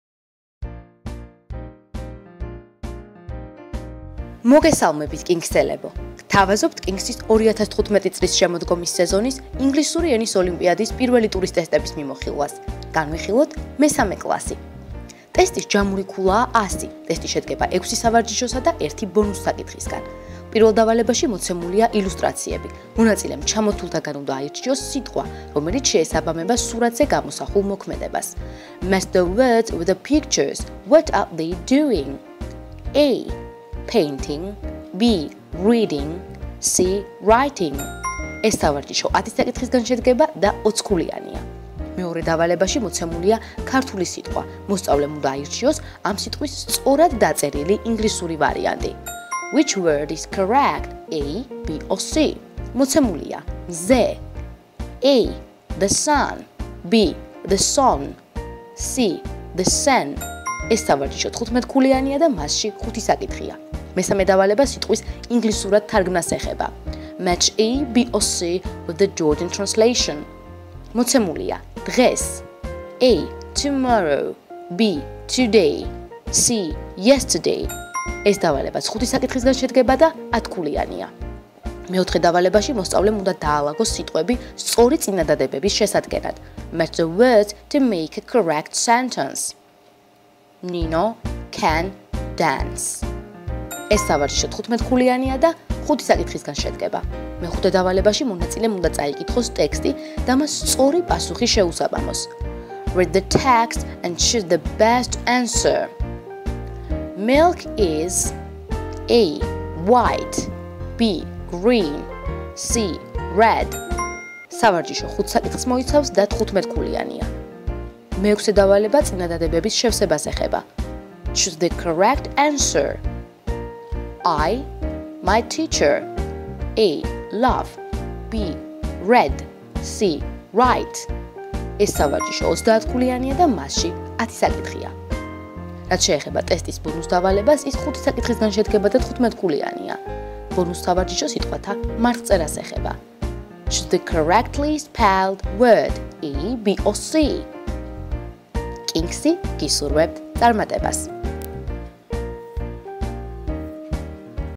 მესამე კლასის ინგლისური ენის ტესტის მიმოხილვა
კინგსის ინგლისური ენის 2015 წლის შემოდგომის სეზონის პირველი ტურის ტესტების მიმოხილვა.